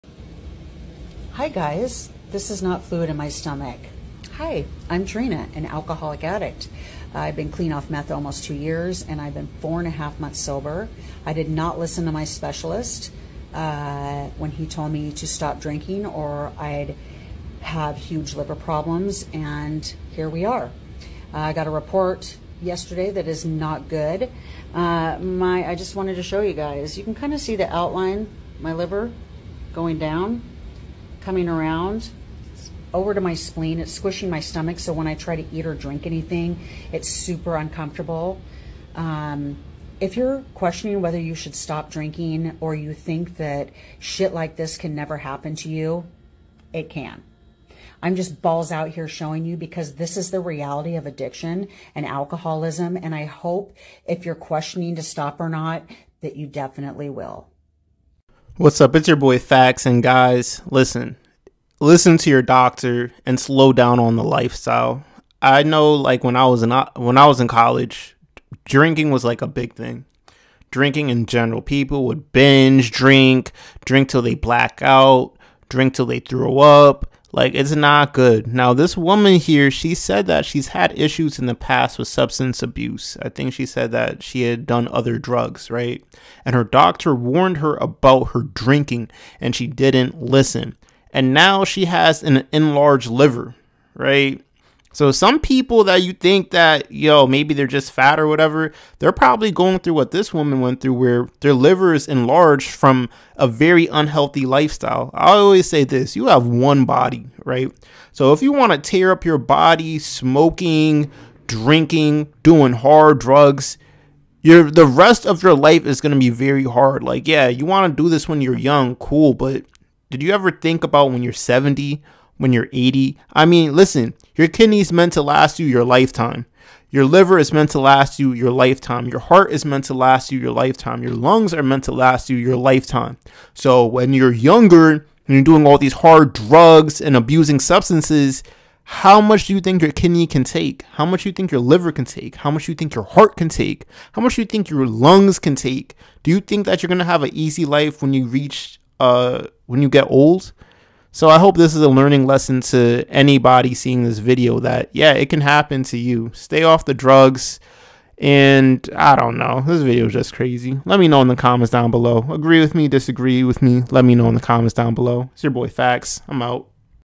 Woman Shows The Effects Of Abusing Alcohol For 28 Years.. Now Suffers From Hepatomegaly
Liquor Is No Joke: Woman Shows The Effects Of Abusing Alcohol For 28 Years.. Now Suffers From Hepatomegaly (Abnormal Enlargement Of The Liver)